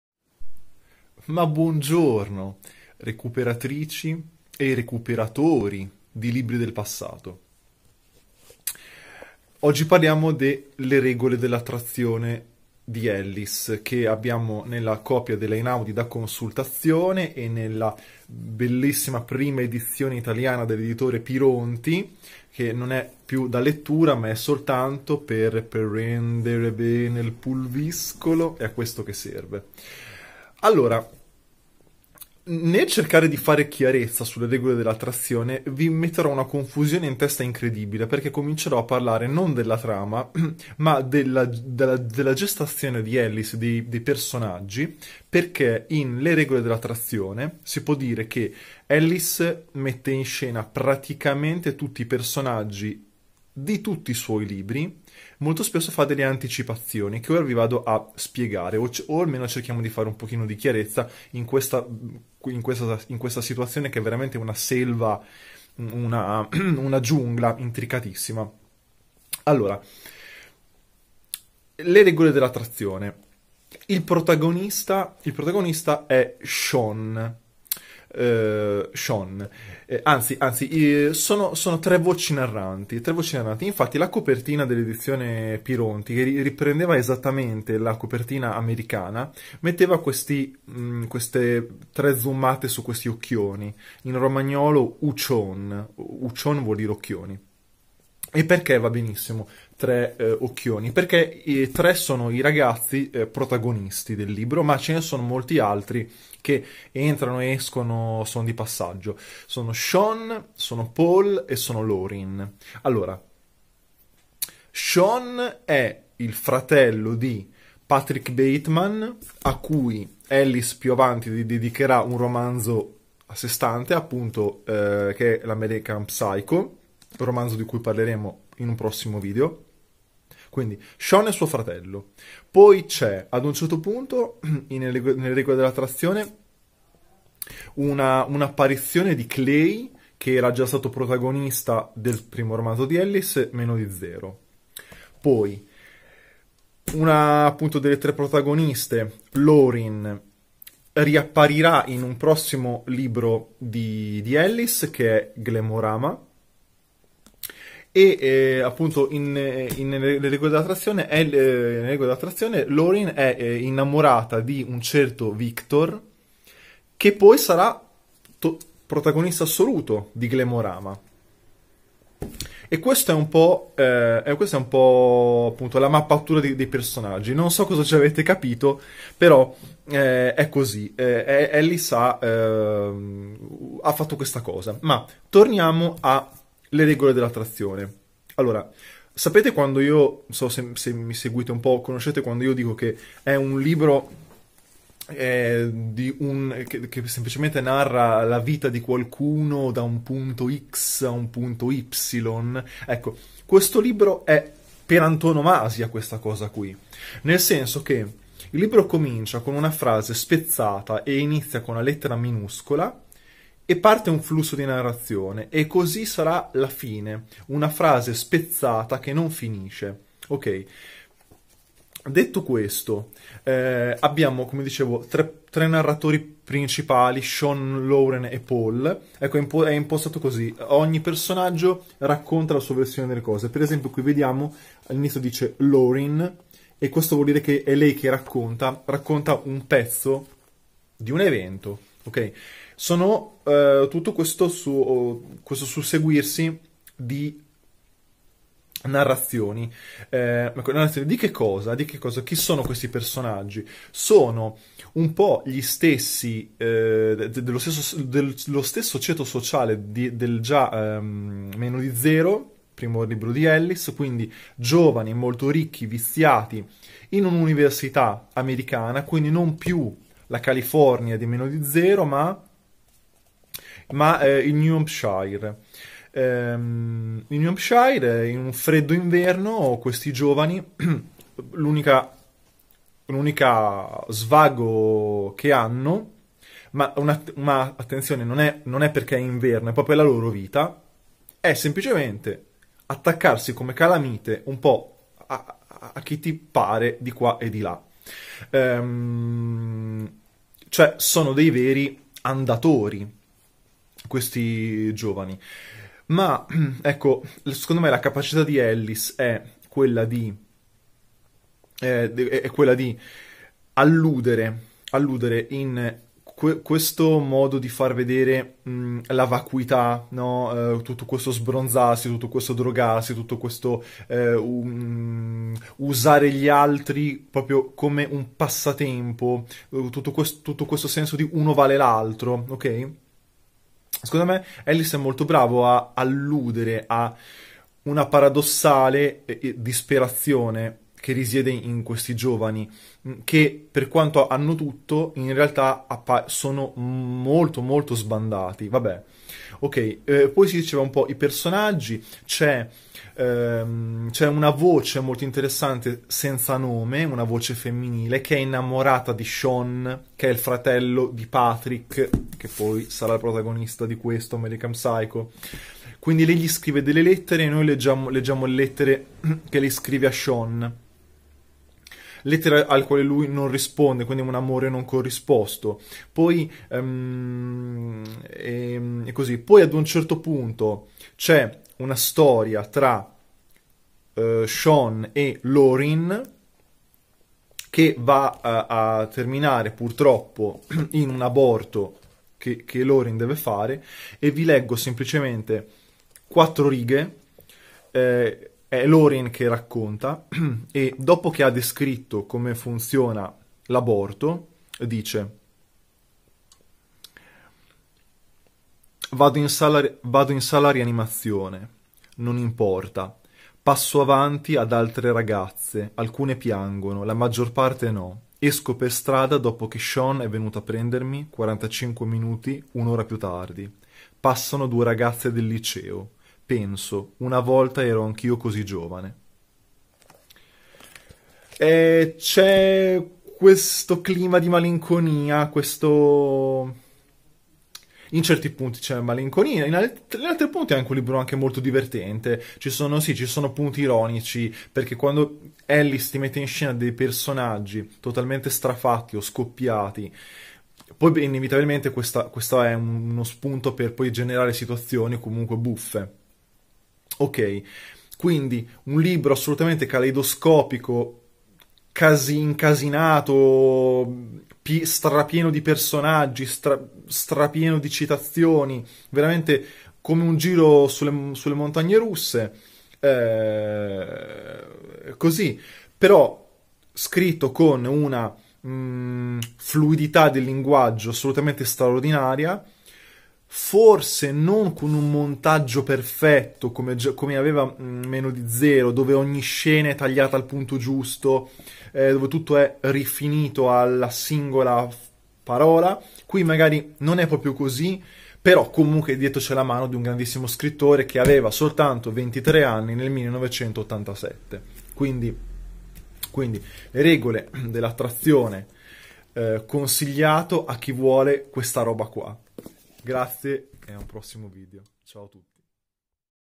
LE REGOLE DELL' ATTRAZIONE di Bret Easton Ellis ( 1987 ) recensione
Questo video fa parte della playlist " BRET EASTON ELLIS "
Recensione di Le regole dell' attrazione di Bret Easton Ellis, pubblicato da Einaudi